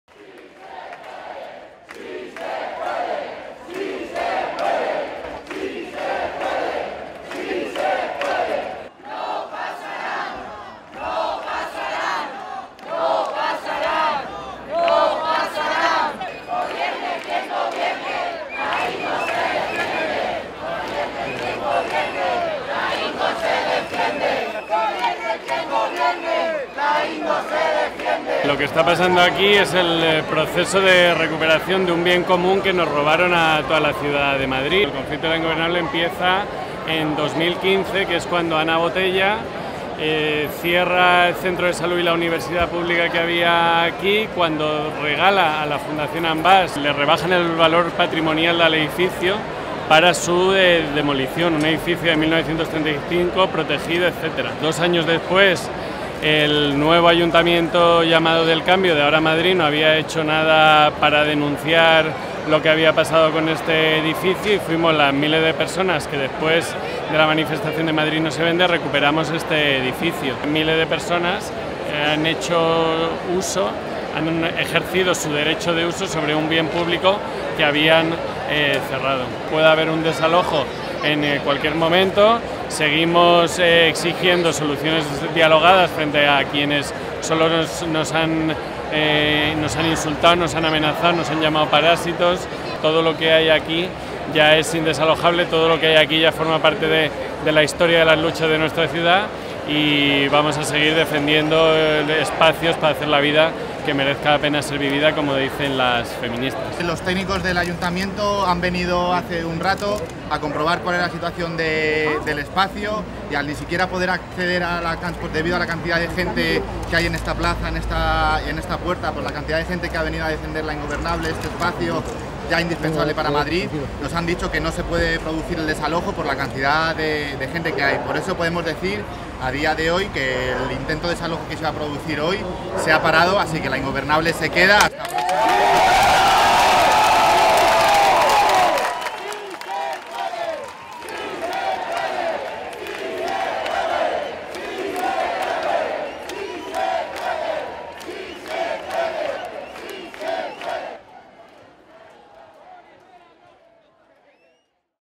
La Ingobernable resiste
Cientos de personas acuden al llamamiento de la asamblea del centro social madrileño La Ingobernable para paralizar el primer intento de cerrarlo por parte del nuevo ayuntamiento.